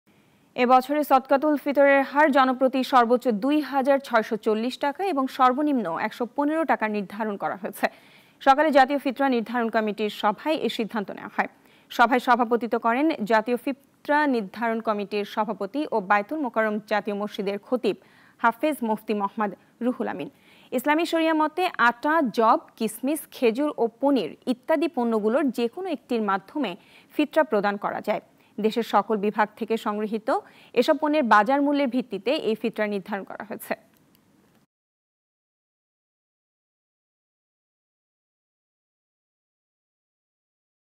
এ বছরের ফিতরা জনপ্রতি সর্বোচ্চ ২৬৪০ টাকা ও সর্বনিম্ন ১১৫ টাকা | Fitra | Nagorik News
এ বছরের ফিতরা জনপ্রতি সর্বোচ্চ ২৬৪০ টাকা ও সর্বনিম্ন ১১৫ টাকা